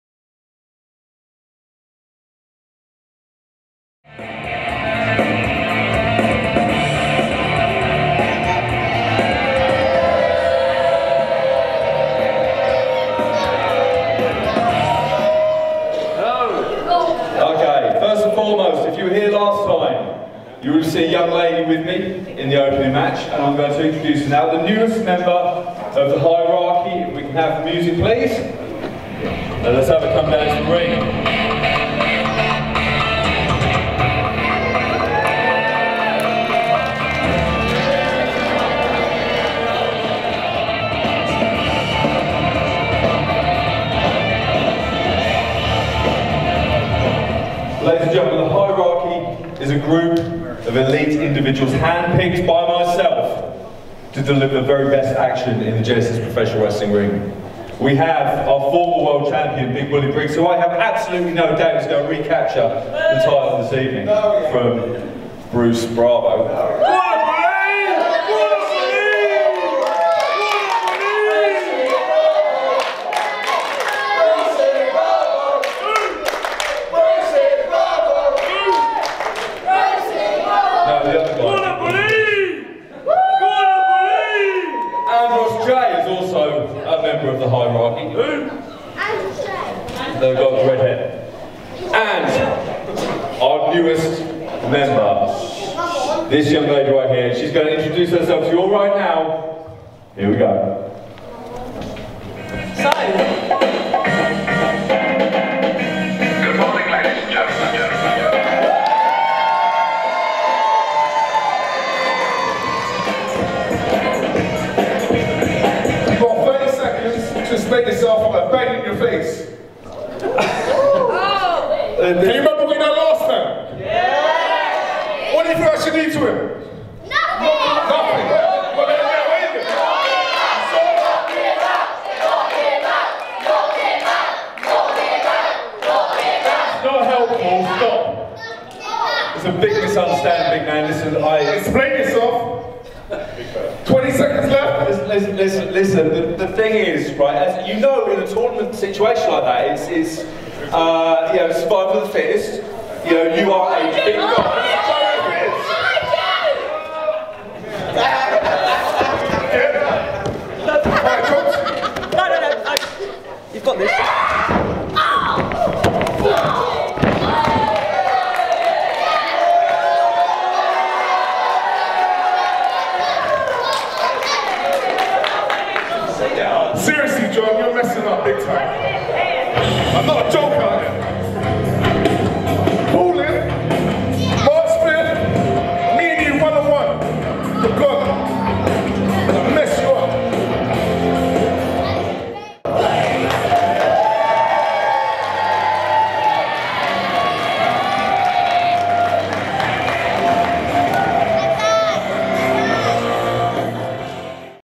Kurupt challenges The Boss Jon Moores
Back in January Jon Moores screwed Kurupt out of his spot in the tournament with the assistance of his new associate Kyra Nixx.
On February 25th at Snodland Community Centre Kurupt called him out for his behaviour and challenged The Boss for a match on Sunday March 5th at Ballin.

The match has officially been signed and now we will see Kurupt and The Boss Jon Moores go head to head on the Adult Show at 7pm at Ballin Nightclub on March 5th 2023.
No doubt the crowd will be rooting for Kurupt to Knock him out on the night.